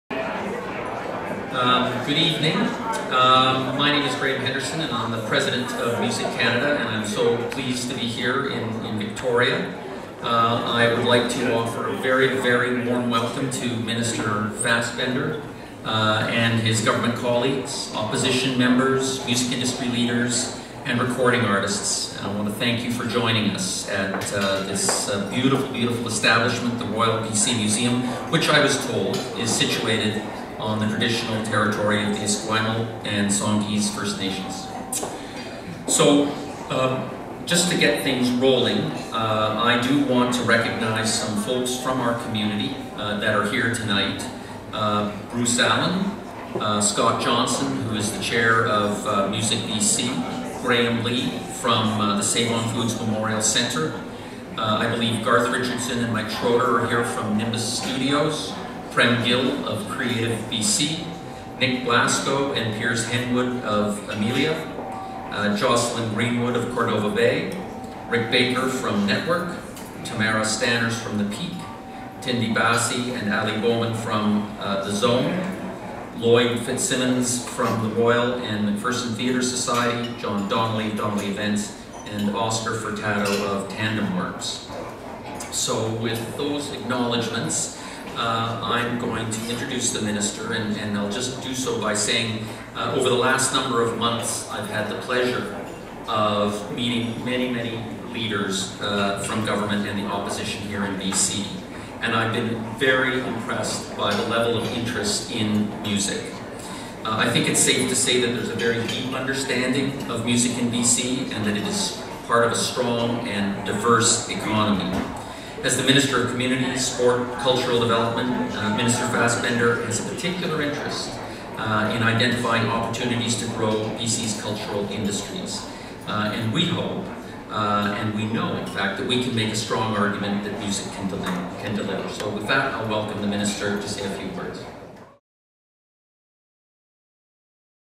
Music Canada's Graham Henderson speaks at BC Music event
A who’s who of BC’s music industry and artists including Bryan Adams, Jesse Roper, members of Current Swell, Cowboy Junkies, Chilliwack, and Spirit of the West came together Monday evening at the Royal BC Museum for an event hosted by Music Canada for provincial politicians and decision makers. Together, they expressed the need for provincial support for music in the form of regulatory reform and reduced red tape, as well as financial support.